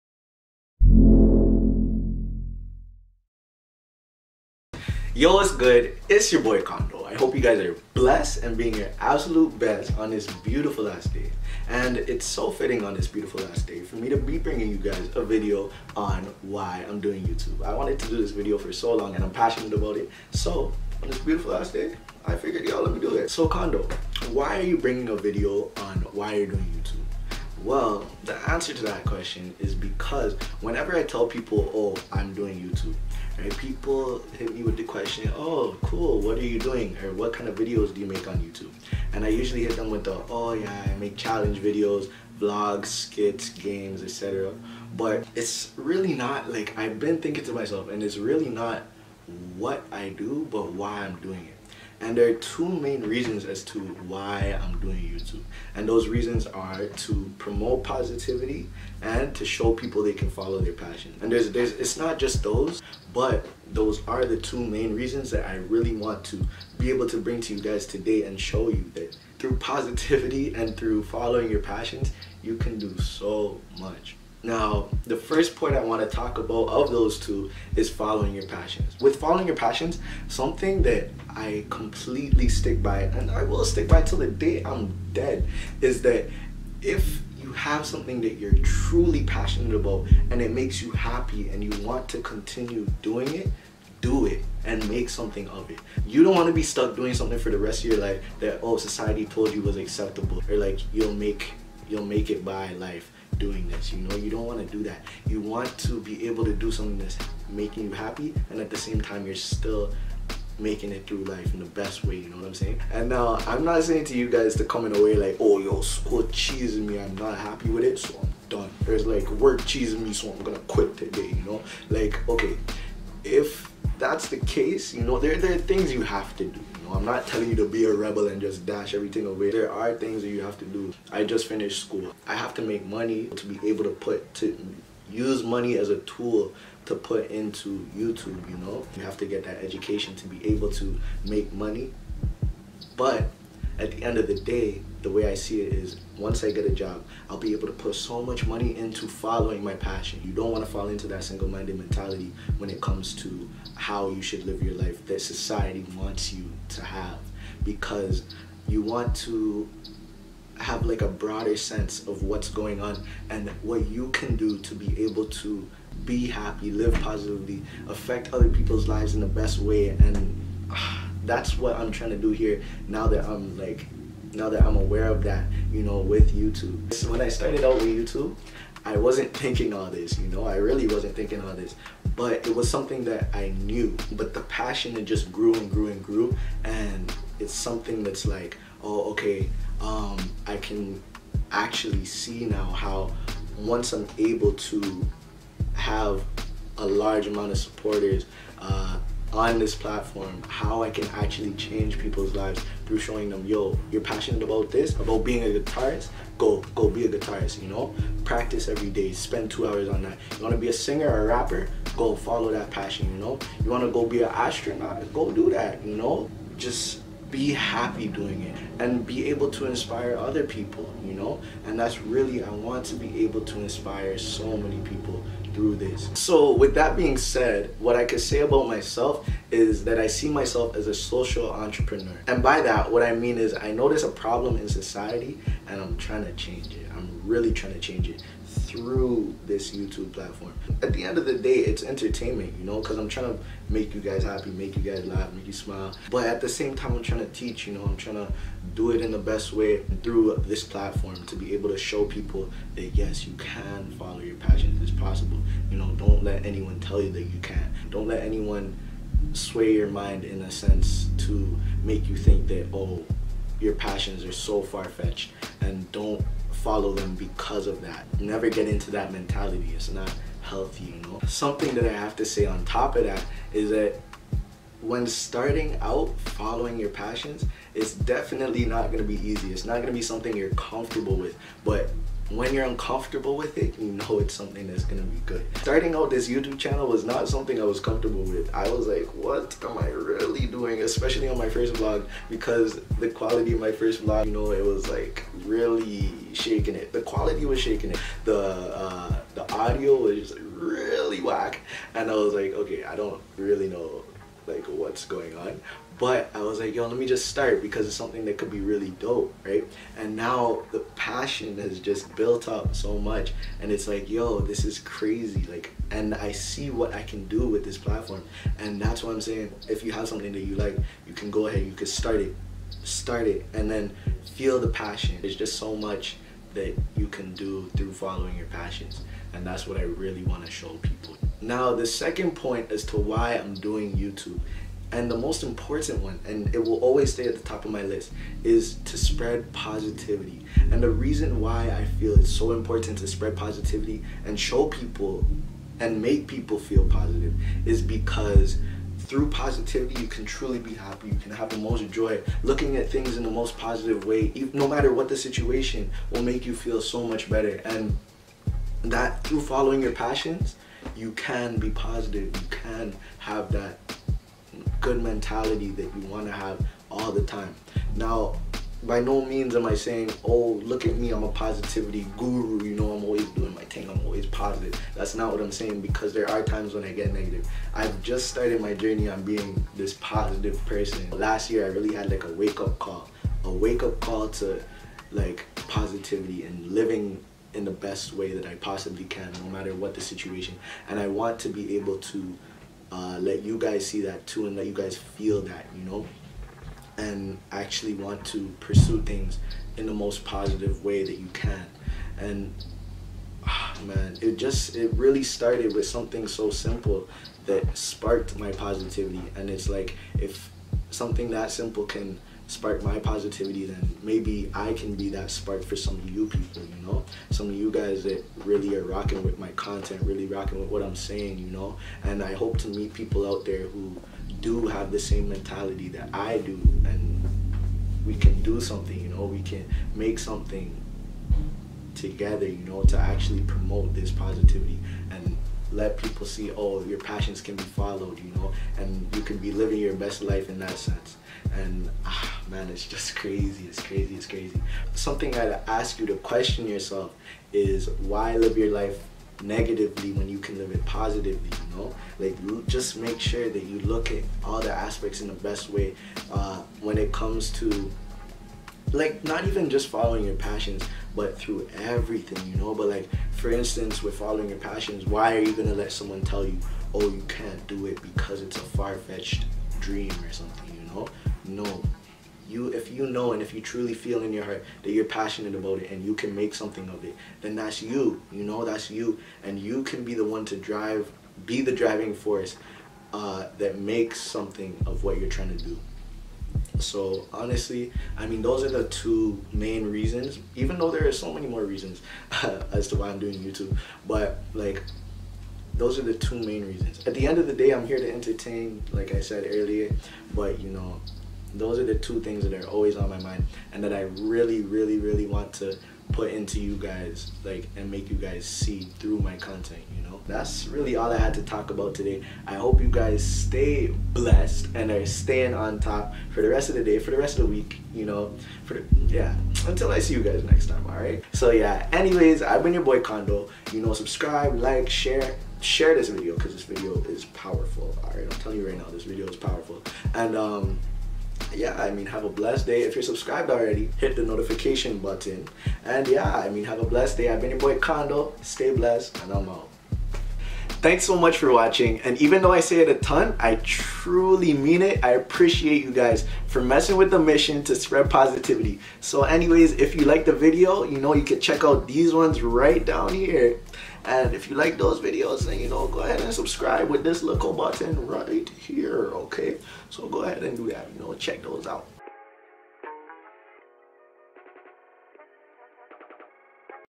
Dive Into My Mind - [Condeau Talks: "Why I'm Doing YouTube"]
There's so much more to why I'm doing YouTube and I really want y'all to know and feel that so hopefully I did a great job with that in this video here :)

Love all the support I've been having so far! It really means a lot to me and I'm so grateful!
Anyways I hope you're having a great day and staying blessed!

Like the beat? Credits to @mellowsins on insta he's a genie wit it!